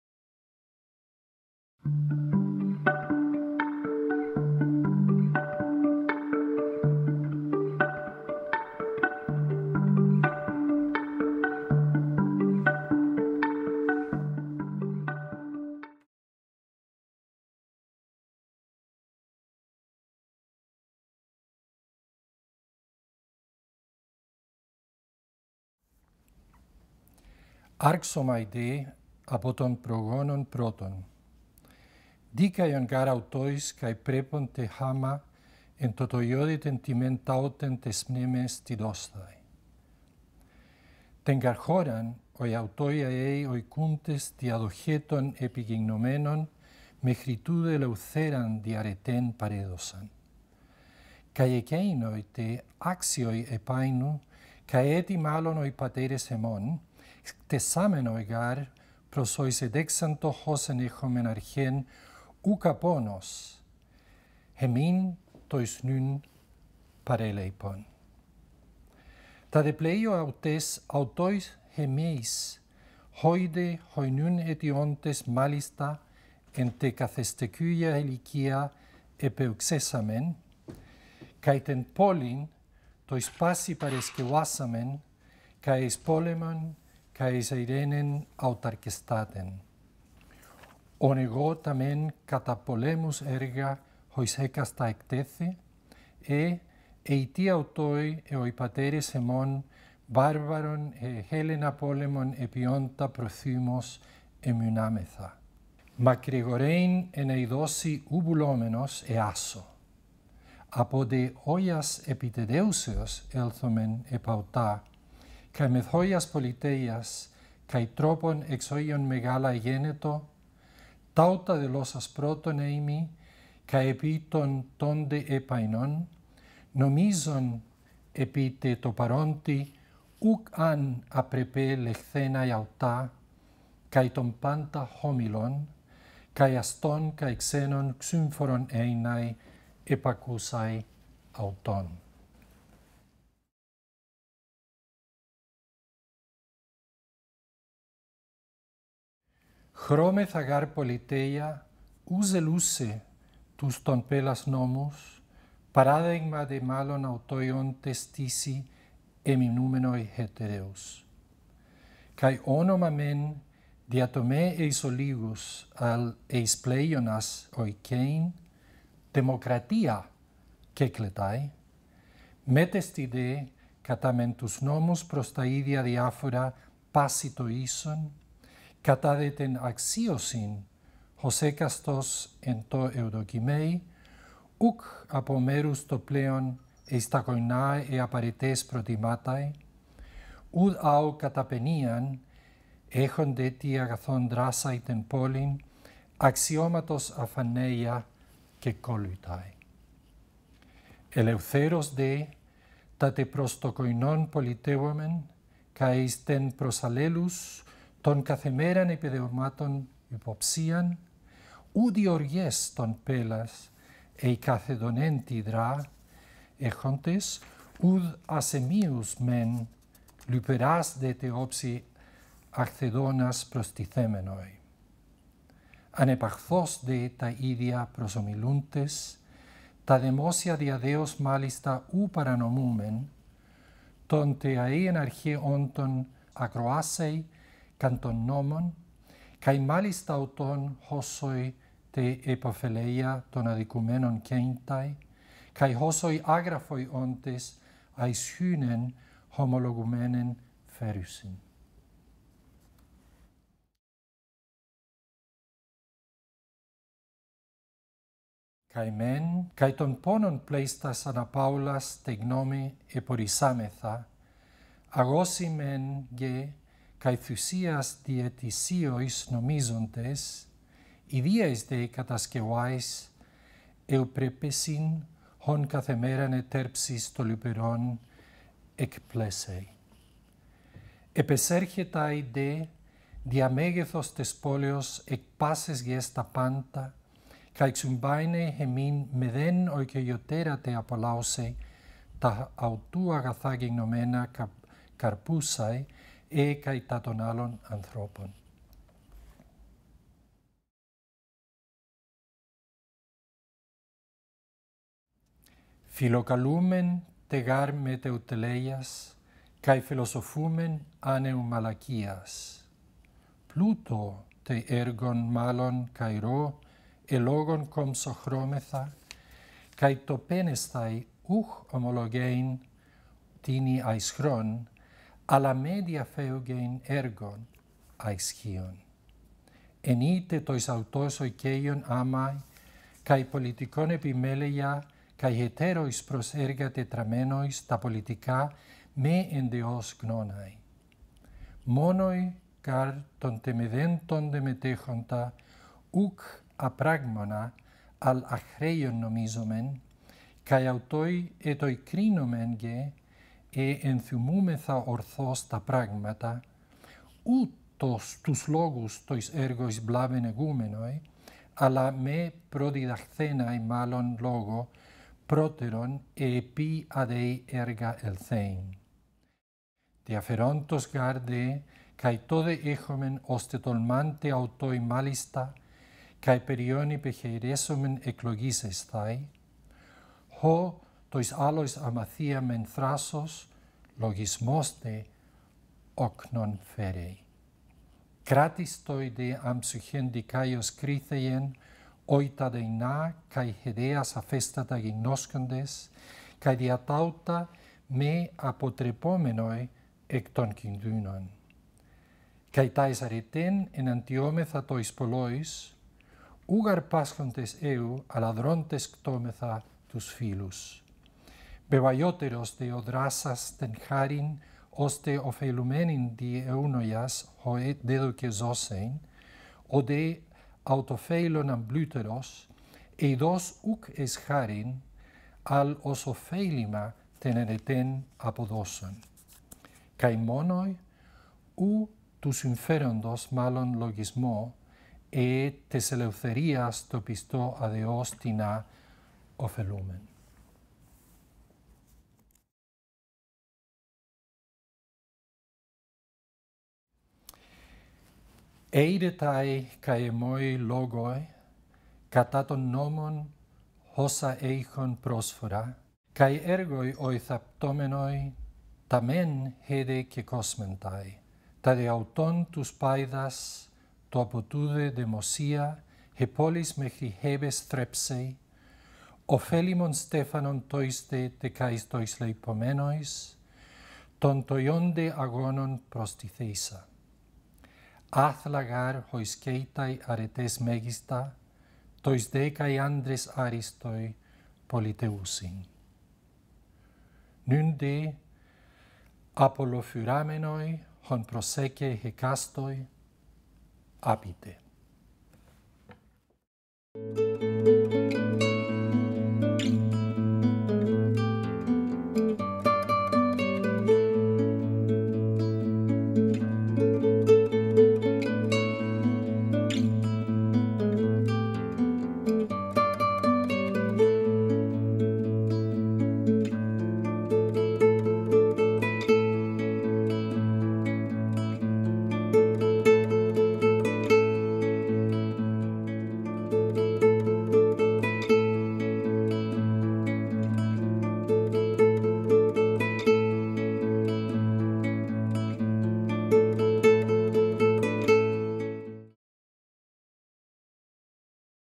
Gómez-Lobo: Tucídides, "Discurso fúnebre de Pericles"
Selección y lectura de textos griegos clásicos por Alfonso Gómez-Lobo (subtitulado).
"Discurso fúnebre de Pericles" de Tucídides, Siglo V a.C.
Traducción de Francisco Rodríguez Adrados.